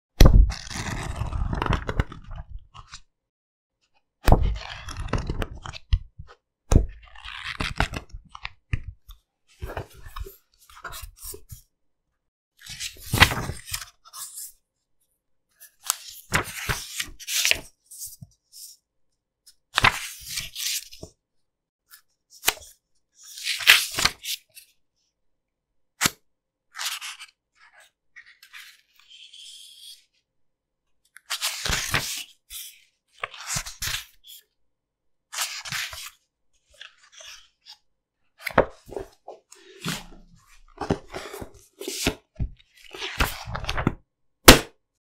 Flipping Pages Sound Effects
0:00 Fast
0:11 Slow
0:25 Random Speed
0:38 Finishing the book